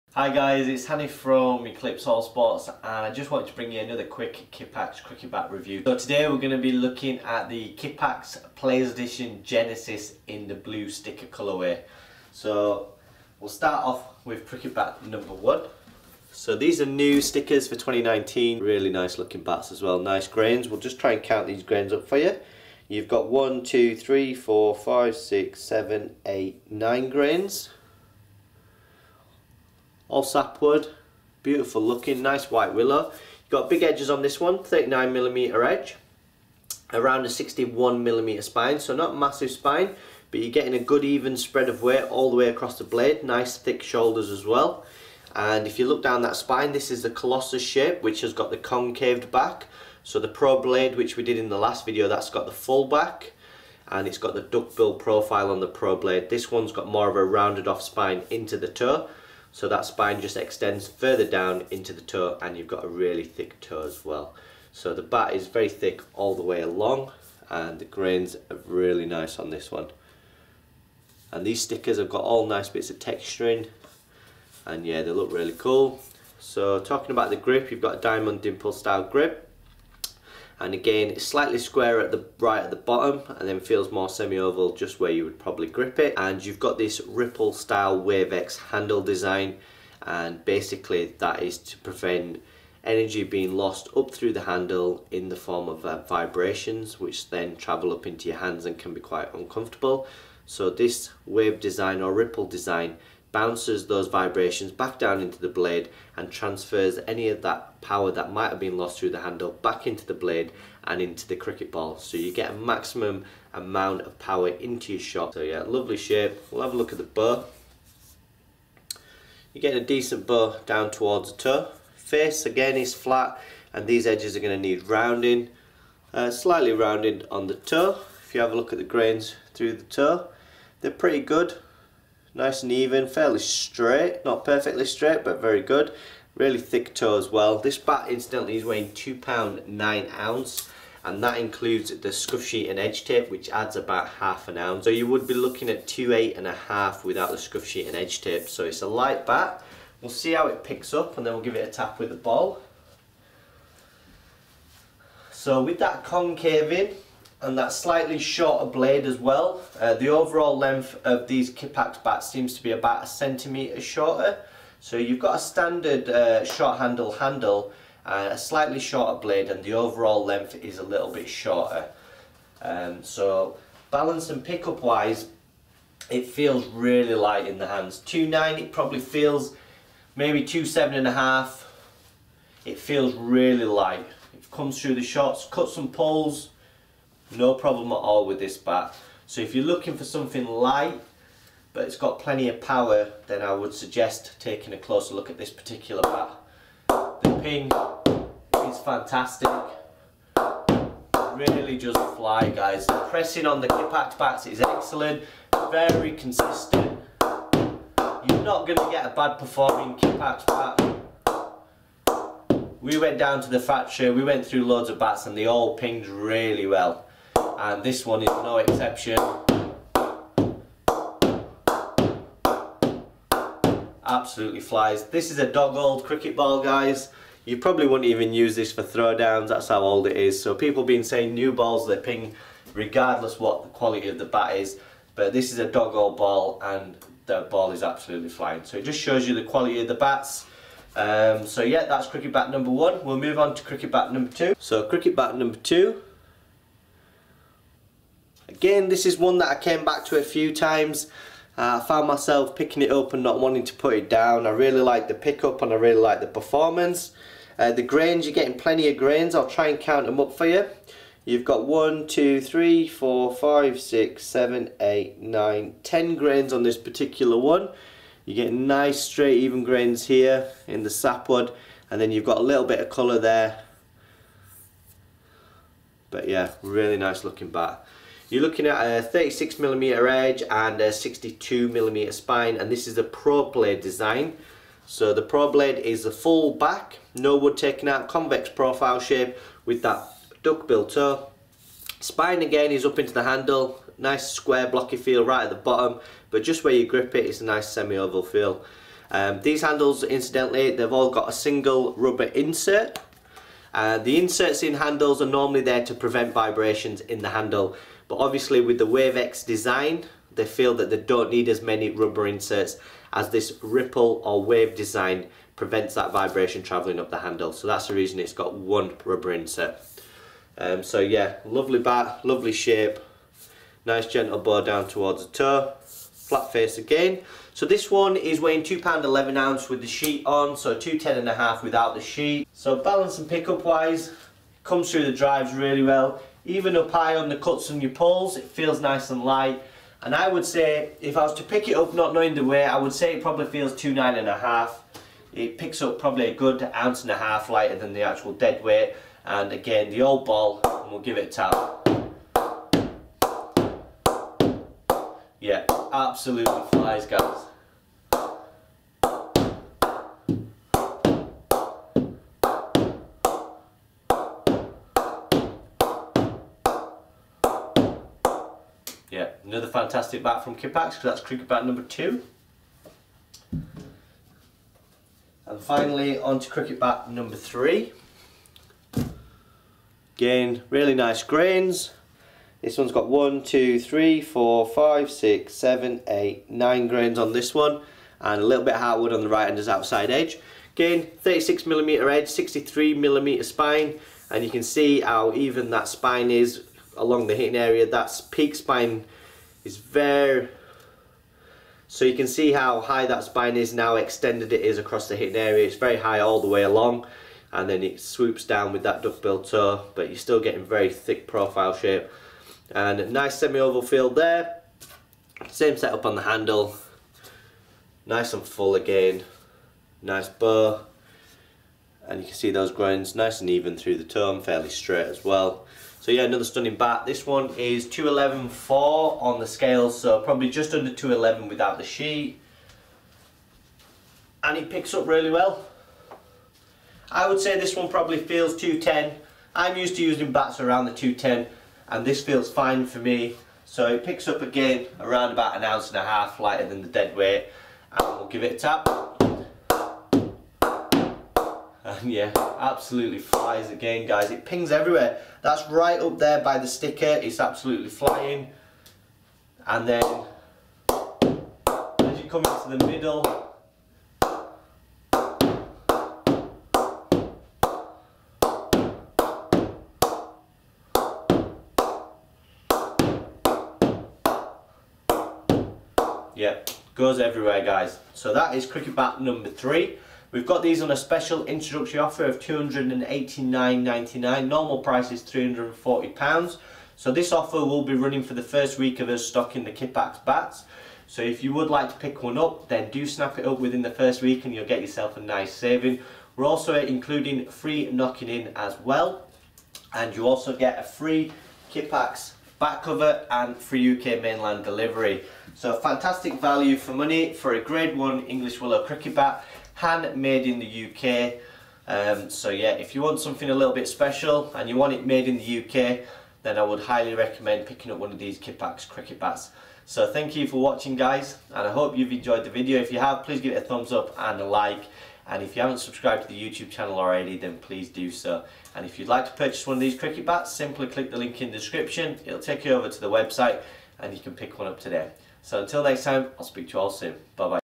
KIPPAX PLAYERS EDITION GENESIS BLUE CRICKET BAT REVIEW
Hi guys it is Hanif from eclipse all sports. I just wanted to bring you a quick video review of the new Kippax cricket bats. Please click the link below to purchase via our website.